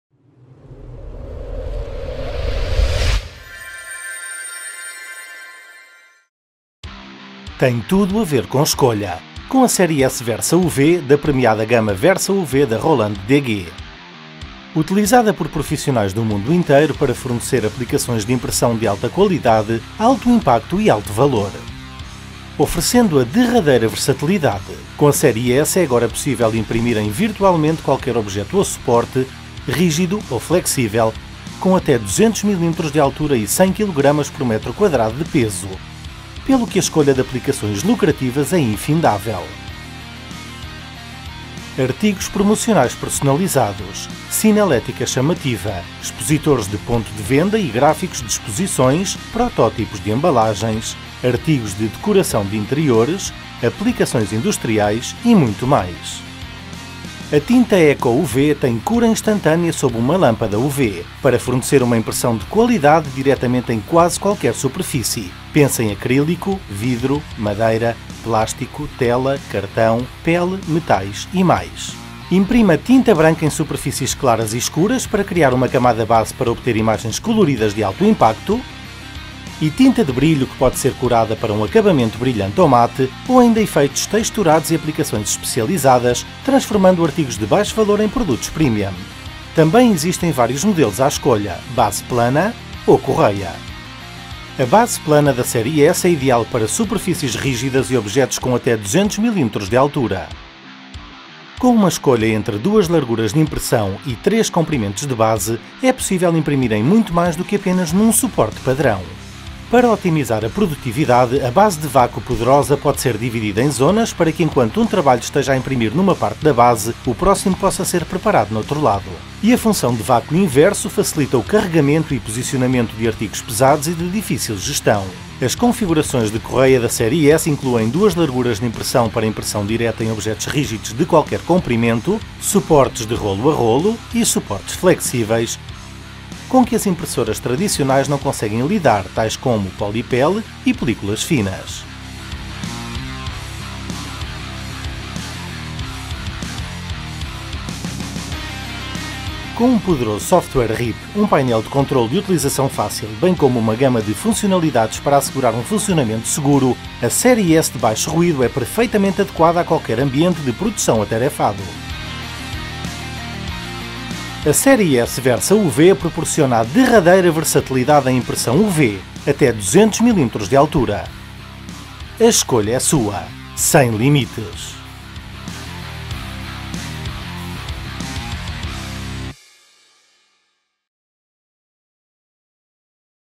Dimatur – Roland – Versa UV Serie S
Com a série S é possível imprimir directamente gráficos de qualidade estonteante, imagens, efeitos texturados e com relevo em praticamente qualquer substrato ou objecto, rígido ou flexível, até 200 mm de altura e 100 kg/m2 de peso.